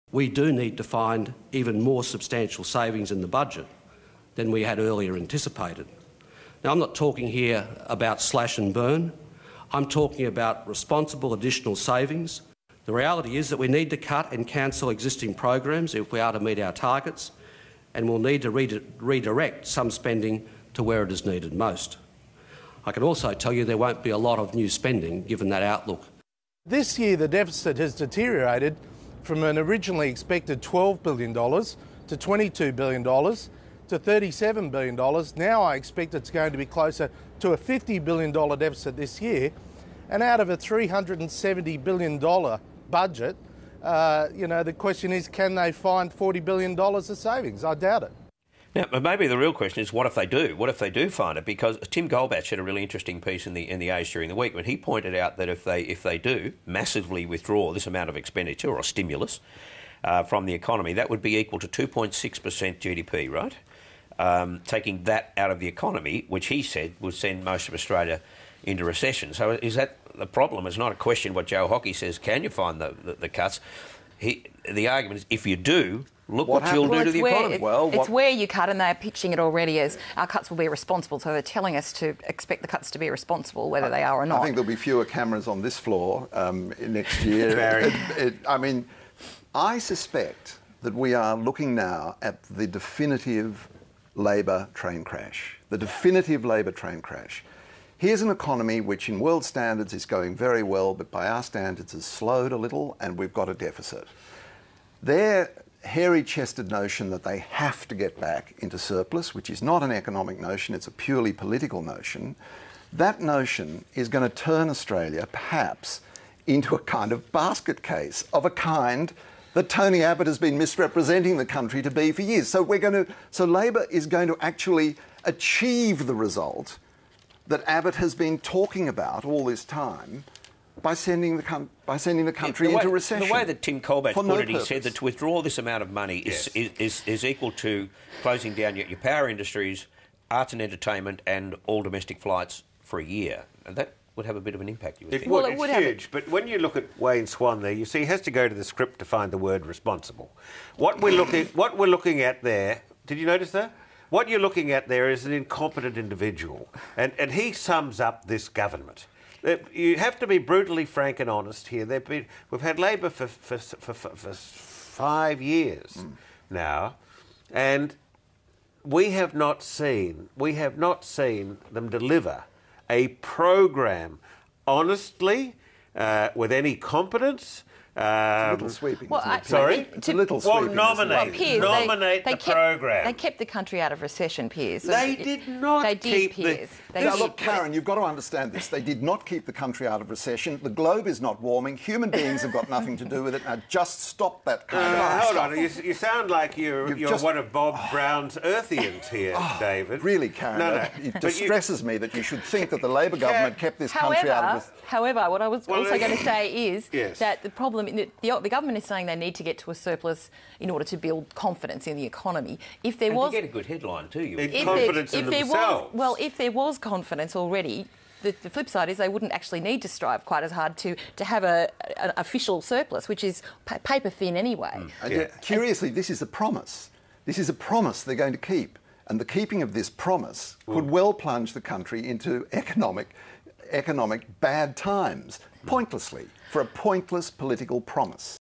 Swan, Hockey bicker over chances of surplus
Shadow treasurer Joe Hockey doubts Wayne Swan can be tough enough to get the Federal Budget back into surplus.